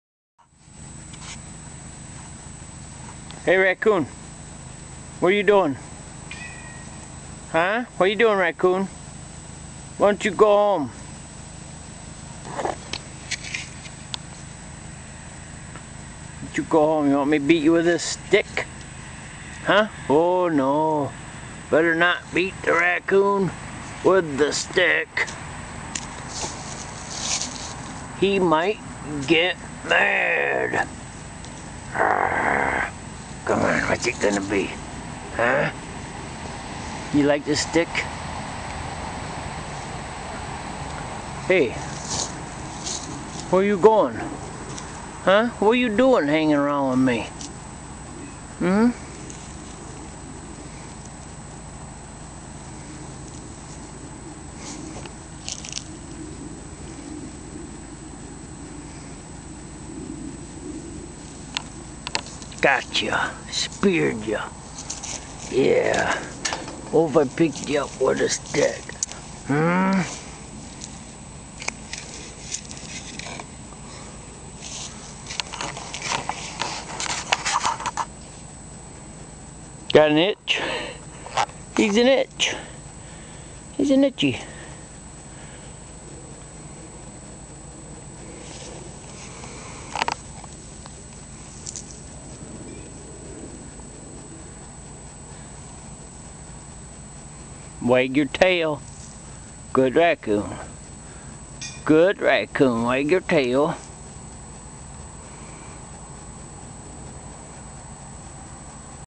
Hey Raccoon
Found footage of some drunk guy messing with a raccoon. "Good raccoon wag your tail".

Hey Raccoon, what are you doing?

Don't beat the raccoon with the stick, he might get mad.

Got ya, speared ya.

Got an itch? He's an itch. He's an itchy.